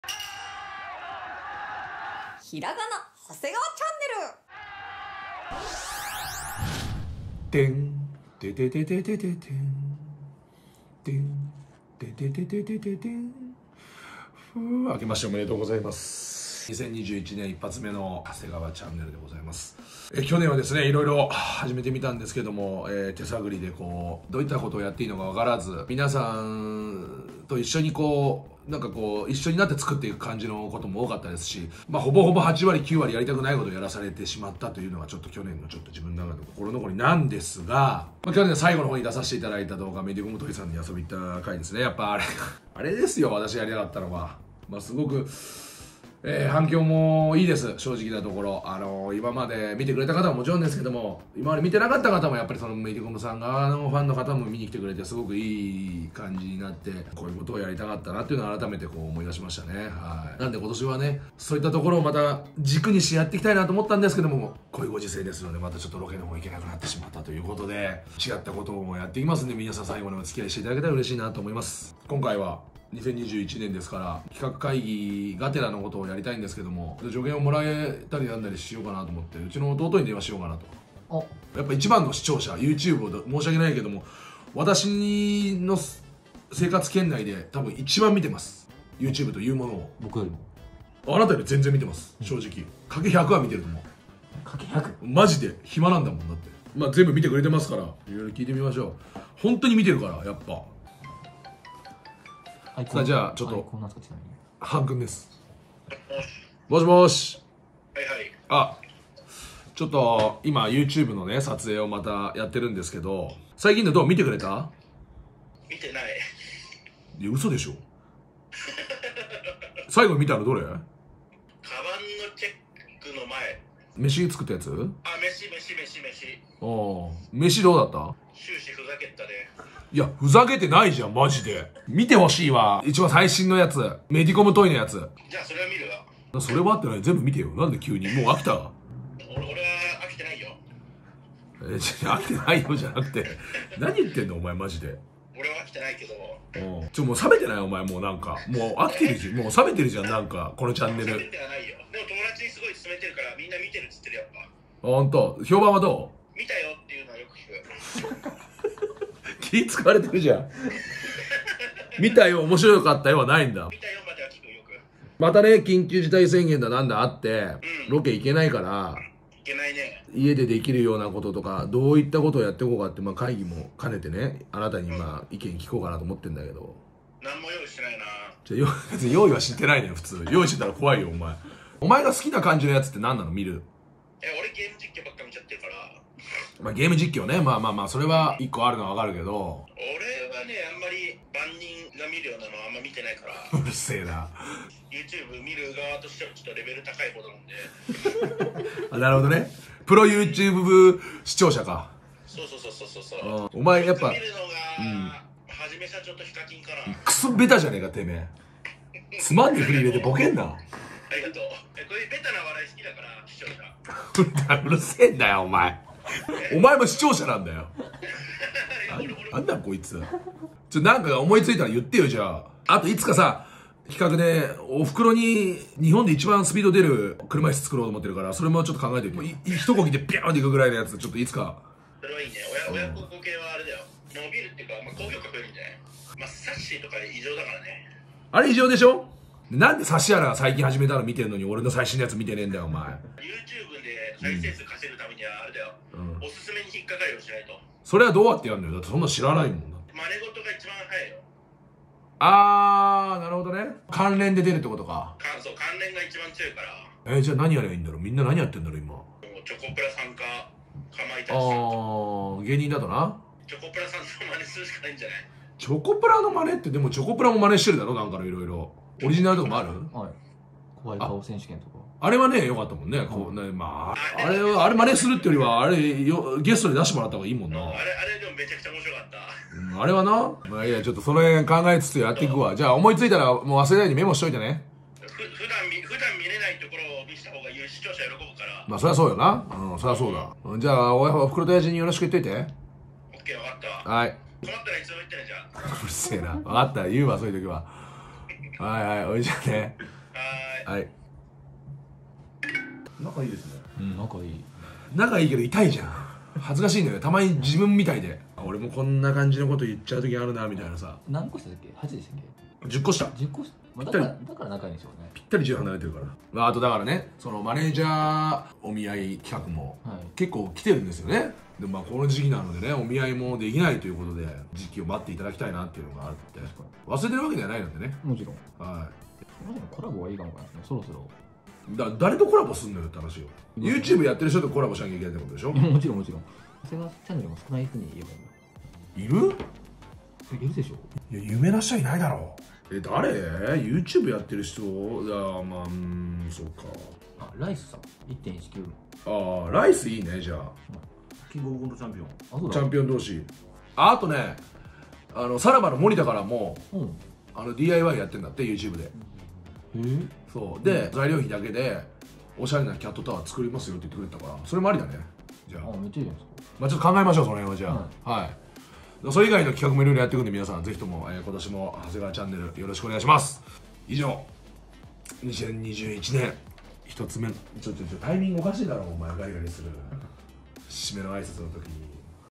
助言を求めた弟から衝撃の告白…
2021年１発目！はせがわちゃんねる始動！

方向性を決めるため、プロYouTubeウォッチャーの実弟・ゆずるに電話！

アドバイスを聞こうとしたゆずるの思わぬ一言に兄はショック…